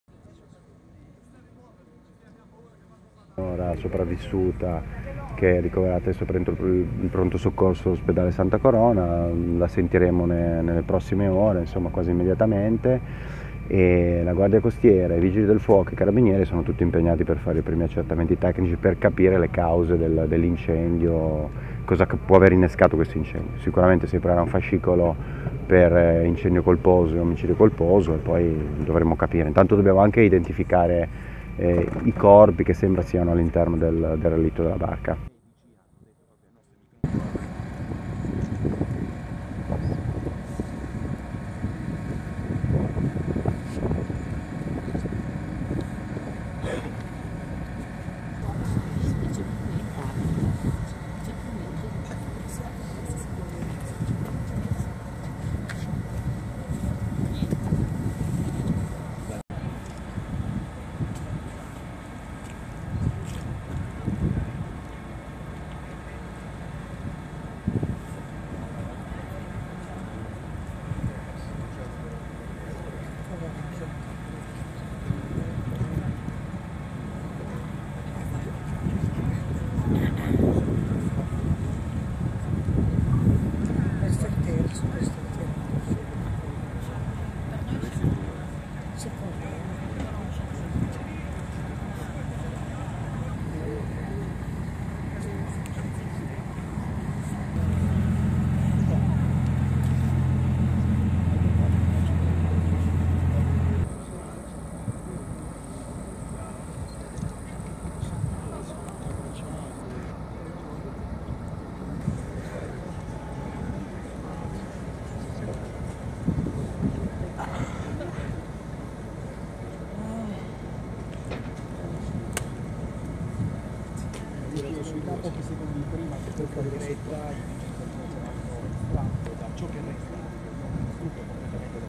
Incendio di uno yacht alla Marina di Loano
Loano. Continuano le indagini, coordinate magistrato Dr. Massimiliano Bolla, per accertare le cause dell'incendio divampato questa mattina presto a bordo di uno yacht ormeggiato alla Marina di Loano [...]